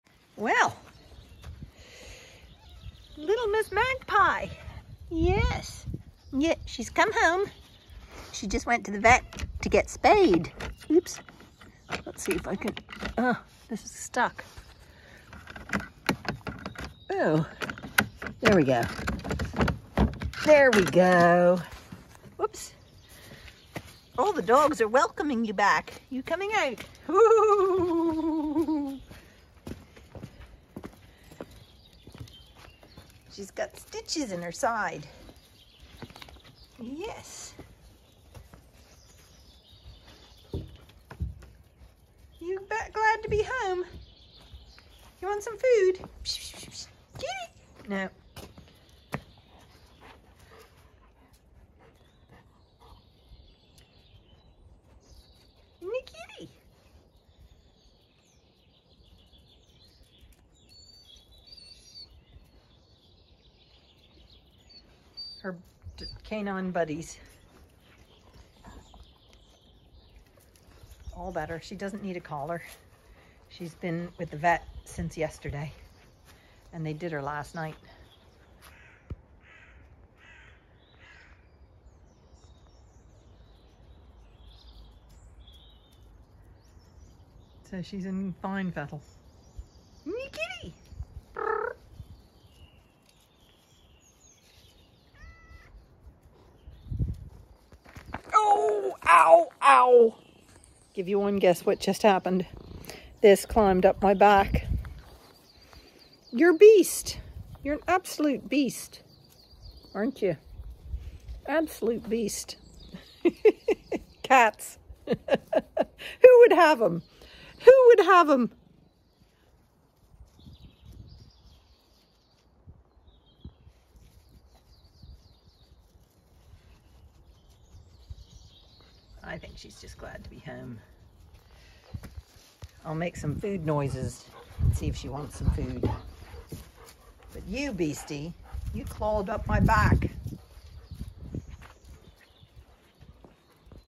Kitty Magpie greeted by canine crew after her spay procedure
We’ve not had kittens born on the farm since I was a child. Only rescued cats have lived here. Magpie continues that tradition.